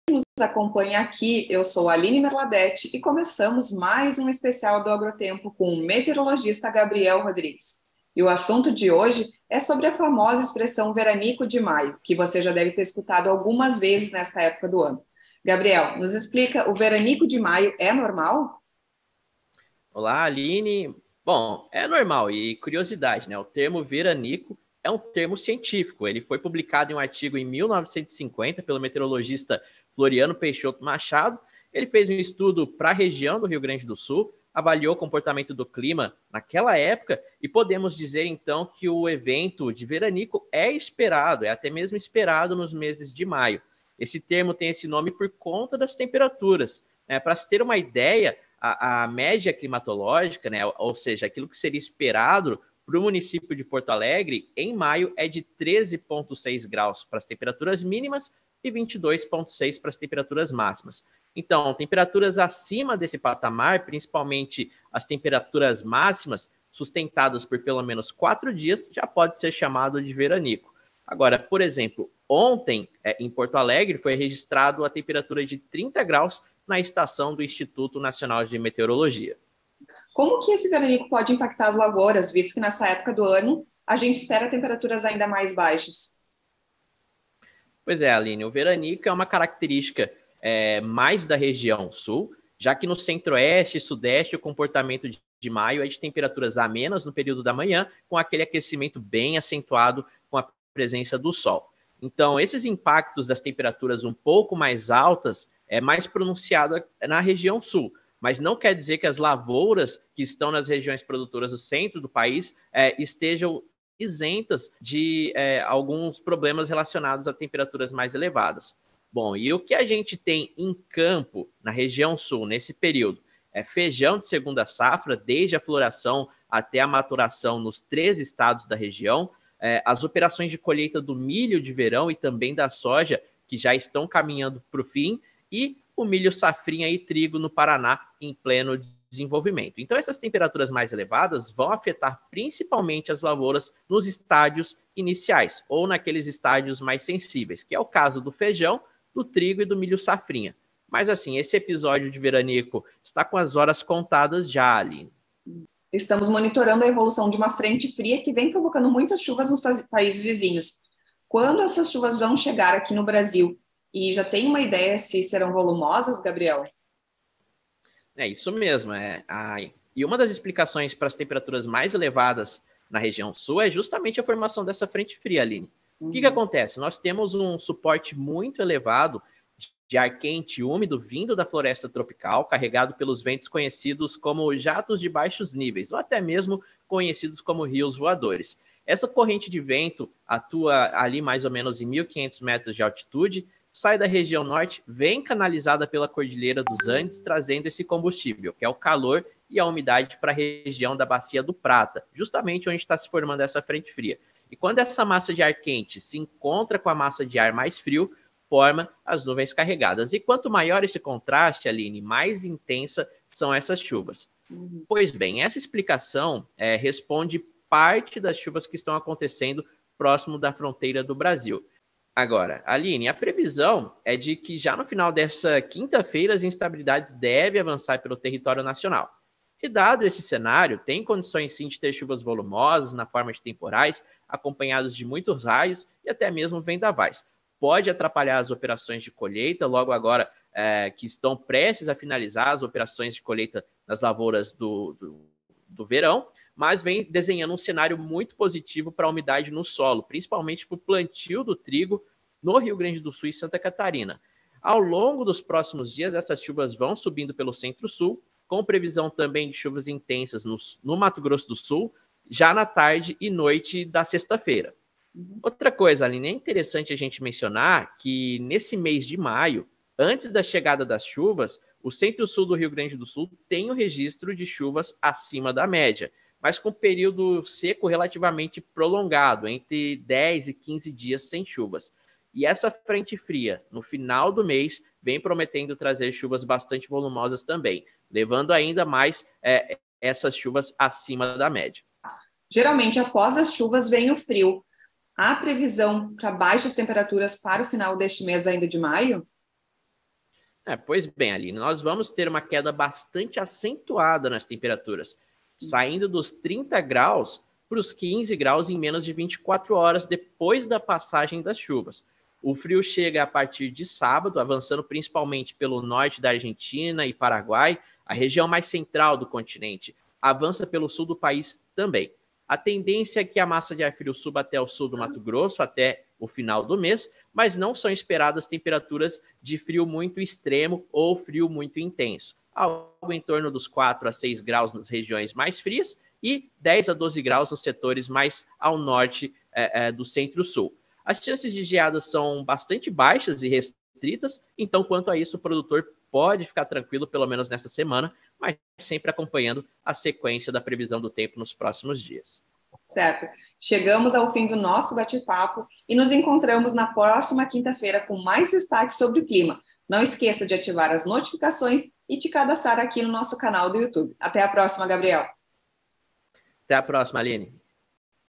Veranico de maio pode impactar as lavouras?
O meteorologista, Gabriel Rodrigues comentou como o veranico de maio pode impactar as lavouras. Assista e saiba quando as temperaturas irão baixar.